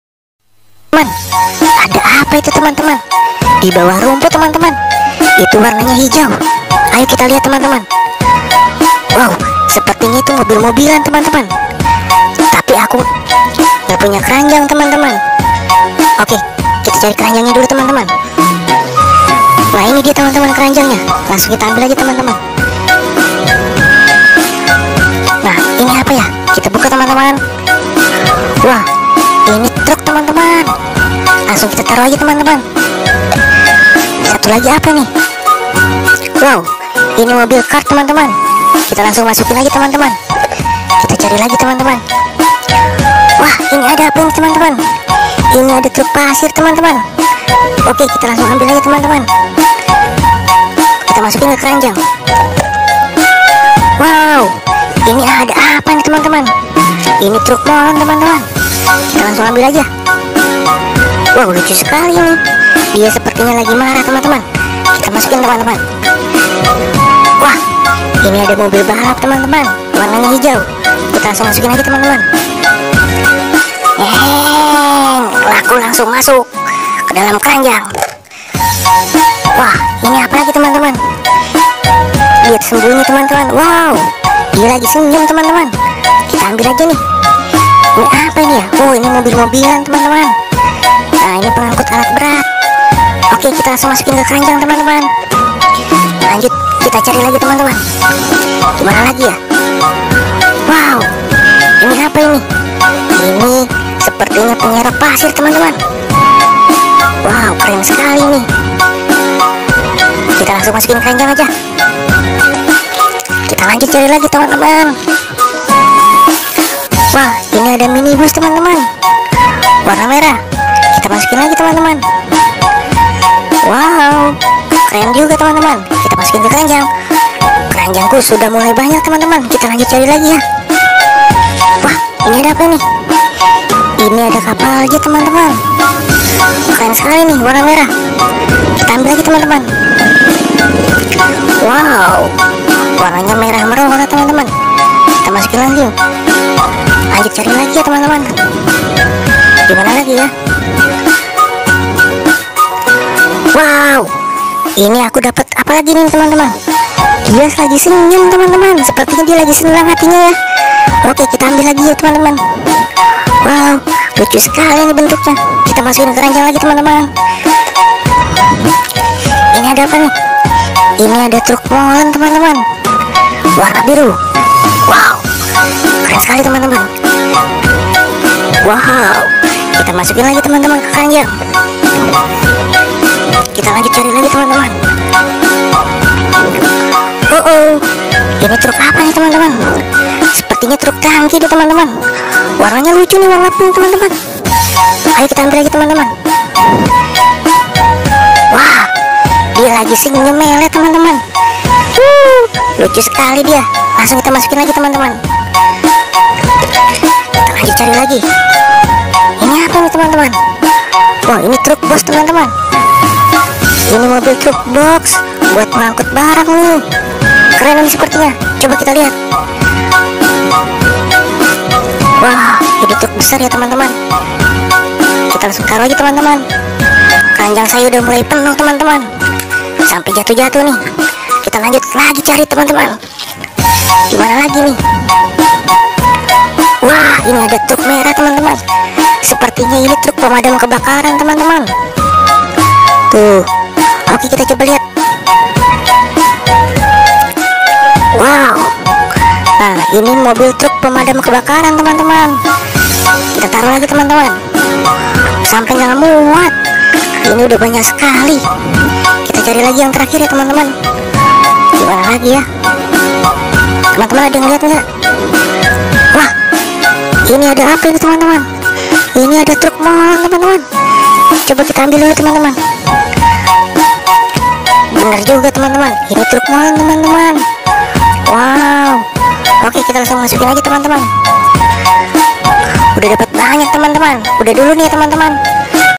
Drama mencari mainan, mobil mobilan, robot robotan, tobot dan ultraman | dratasdut25
Drama mencari mainan, mobil mobilan, robot robotan, tobot dan ultraman

Hallo teman teman di video ini aku mencari harta karun macam-macam mainan di lumpur atau di sawah mulai dari ultraman, mobil mobilan, masak masakan, dokter dokteran, robot robotan, bus tayo, spongebob, ultramen, tobot, spiderman, mobil truk, ultraman zero, ultraman ginga dan masih banyak lagi teman teman.

dratasdut25